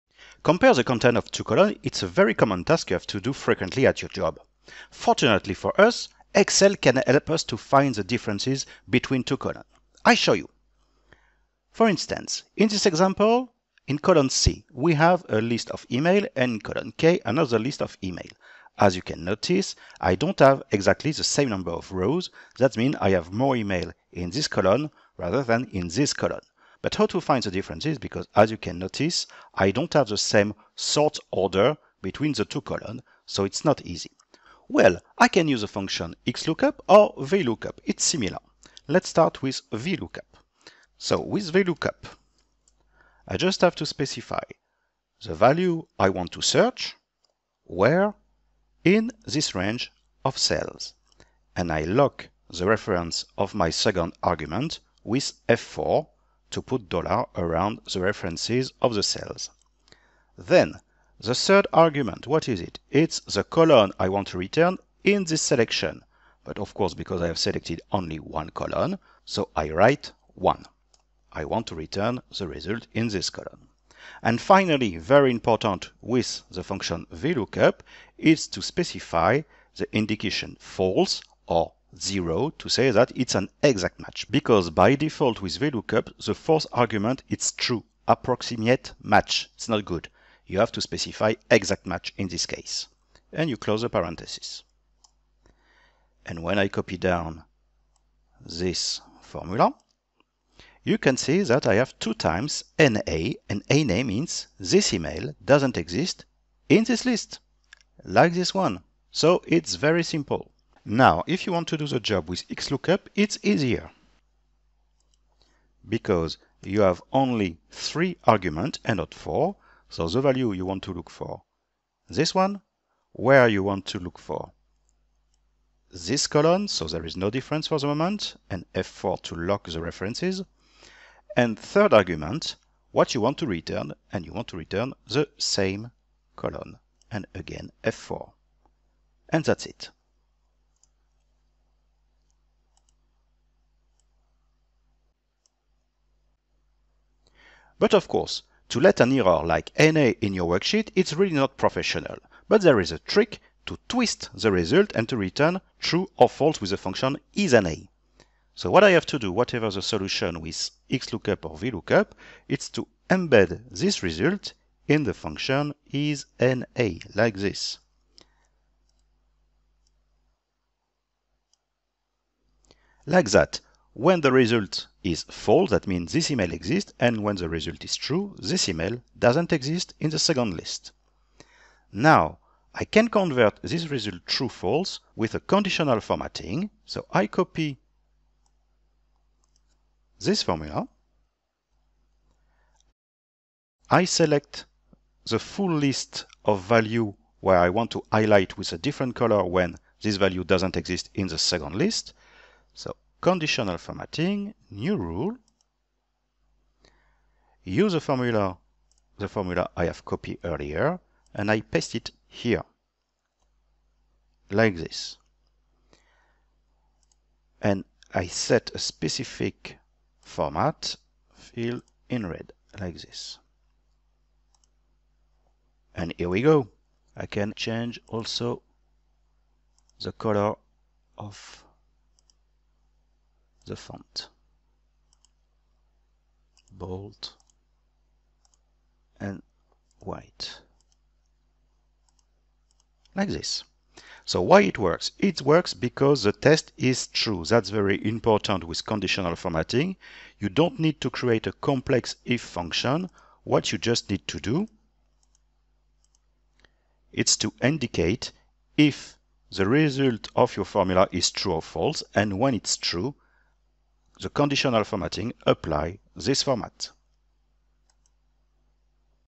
How to Compare two columns with Excel
You can easily compare two columns in Excel with the function VLOOKUP or the function XLOOKUP. You can also add a conditional format to highlight the differences

00:00 Intro
00:34 VLOOKUP function
01:50 XLOOKUP function
02:27 Remove the N/A with ISNA
03:01 Change the color of the cells